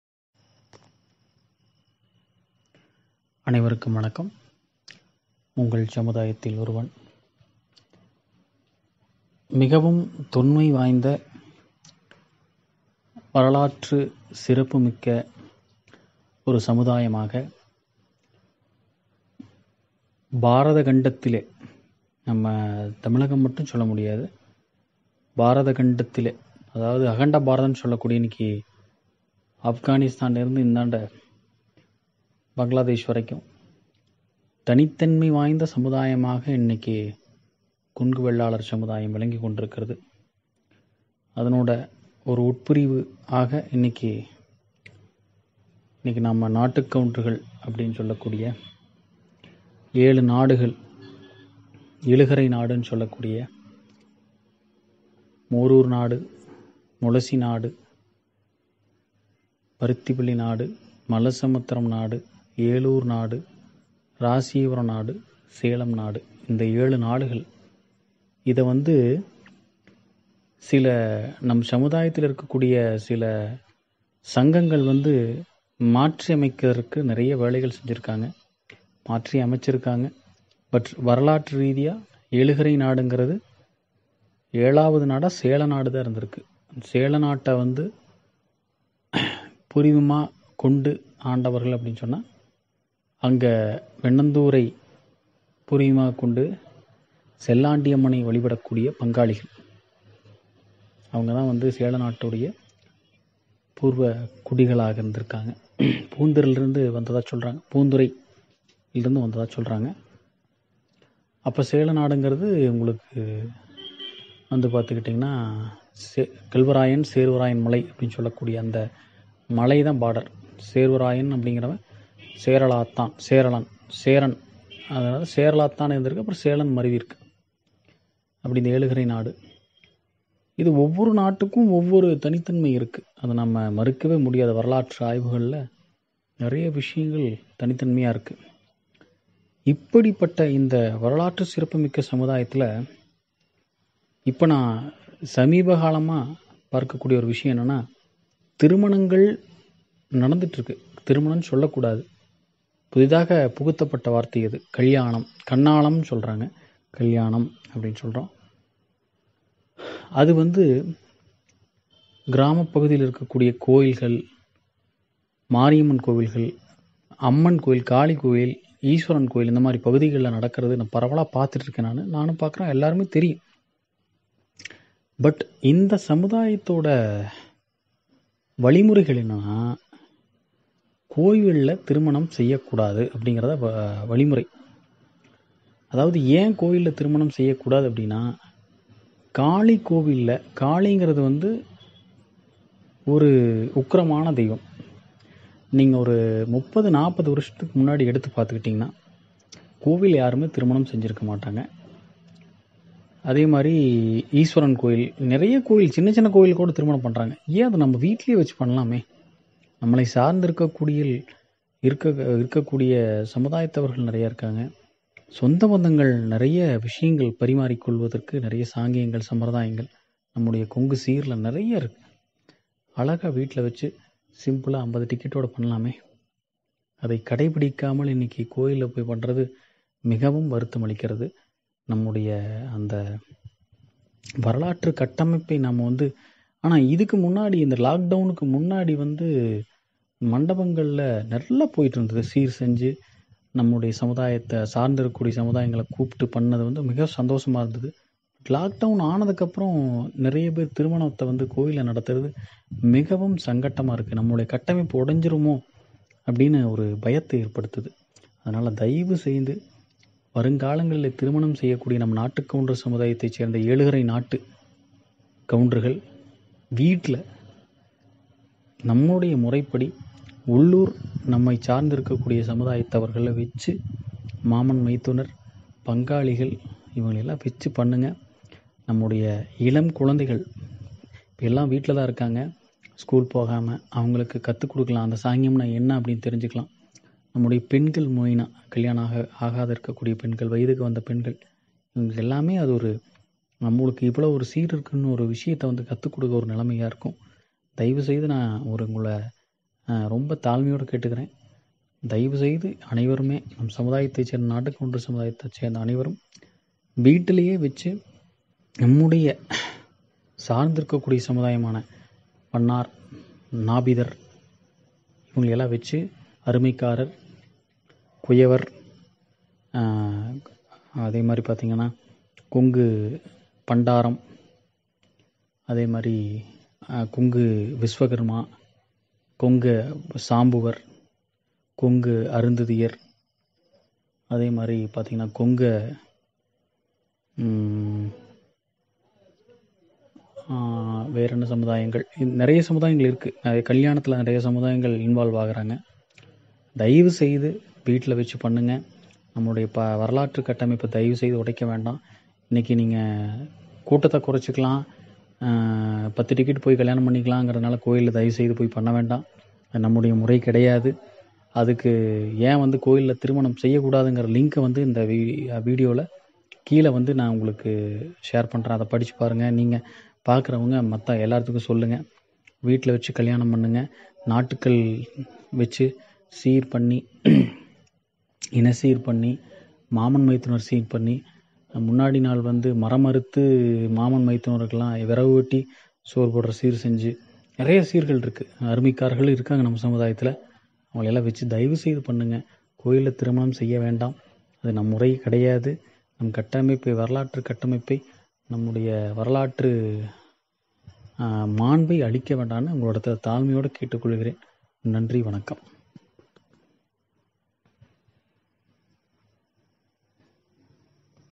மணமகள் வீட்டில்தான் திருமண நிகழ்ச்சி நடக்க வேண்டும்
சூரியன், சந்திரன் உள்ளவரை கொங்கு 18 குடிகள், 64 ஜாதிகளையும் நாங்கள் காப்போம் என்று சோழ மன்னனிடம் நாம் சத்தியம் செய்துகொடுத்துள்ளோம்.

முடிந்தவரை கோவிலில் திருமணம் செய்வதை தவிர்த்து நம் முறைப்படி மணமகள் வீட்டில் செய்வோம்.